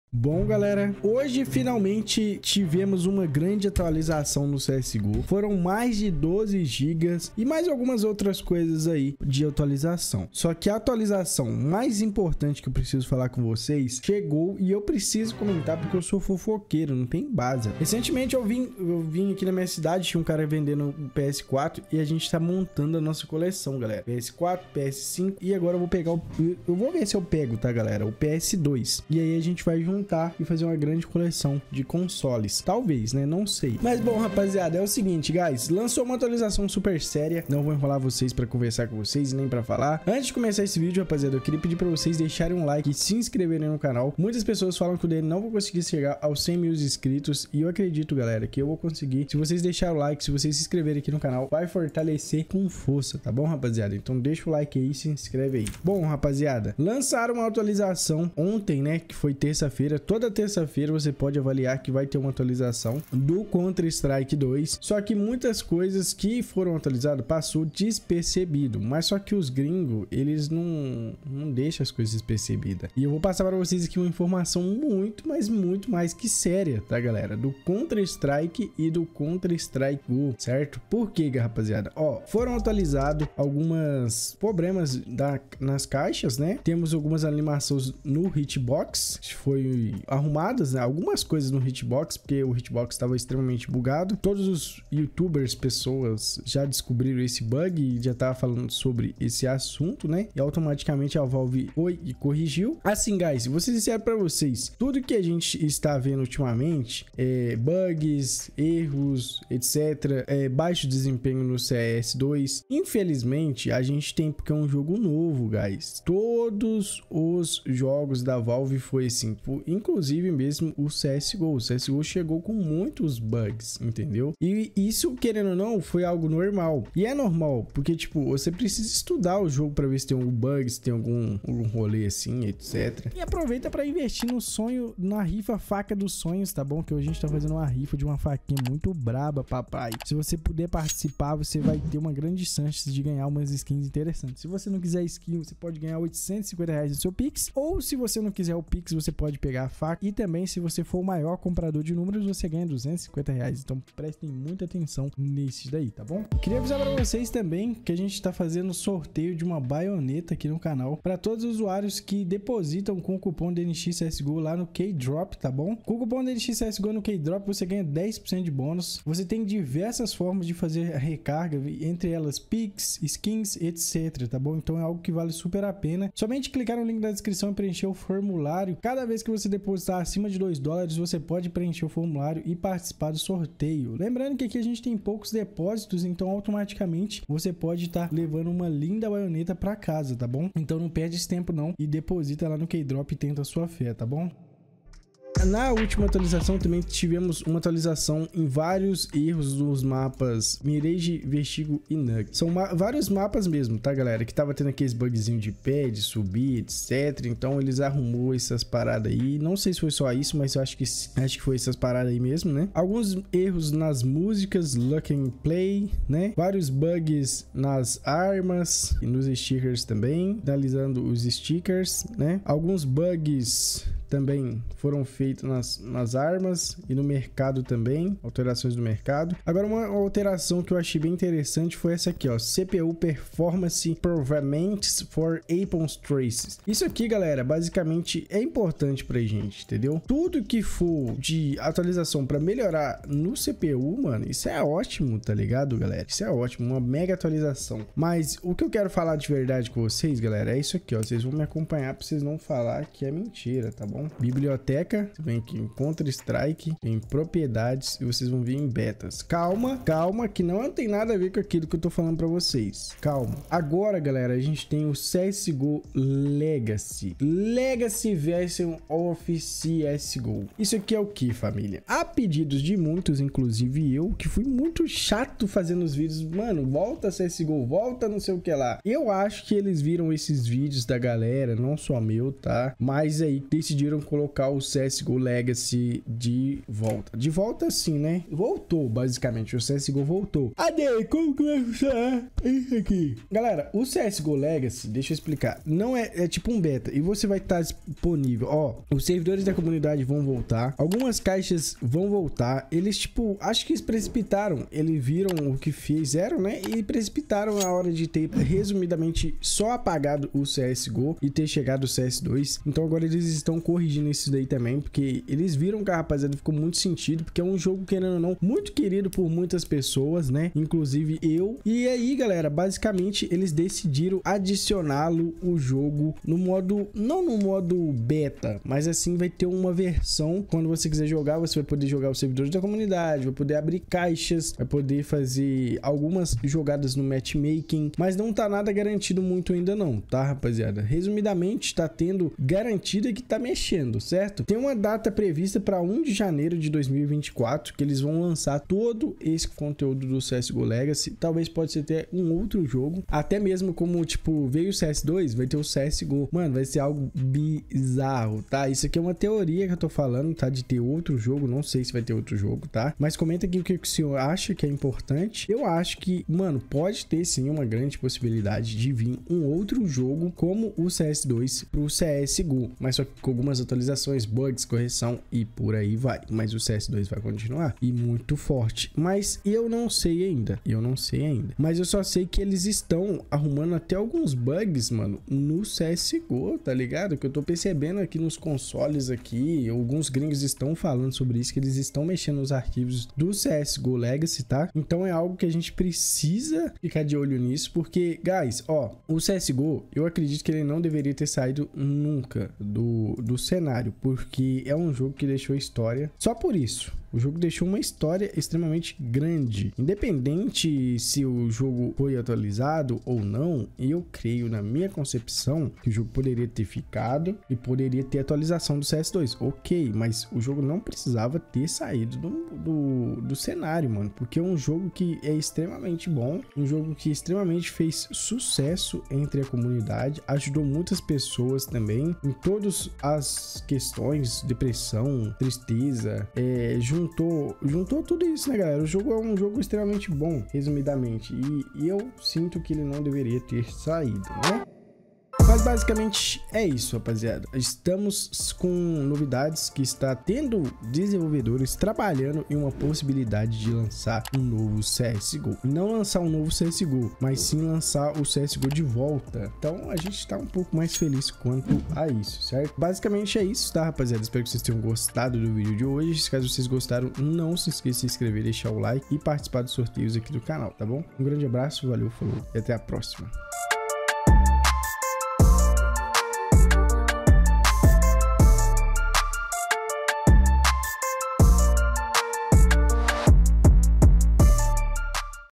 CS:GO está de VOLTAAA. CS:GO LEGACY CHEGOU, possível fim do CS2?
CS2 corre um GRANDE RISCO? Será o fim do CS, vamos Vê UM POUCO sobre essa grande atualização do JOGO MAIS conhecido de FPS

CS:GO está de VOLTAAA. CS:GO LAGACY CHEGOU, possível fim do CS2?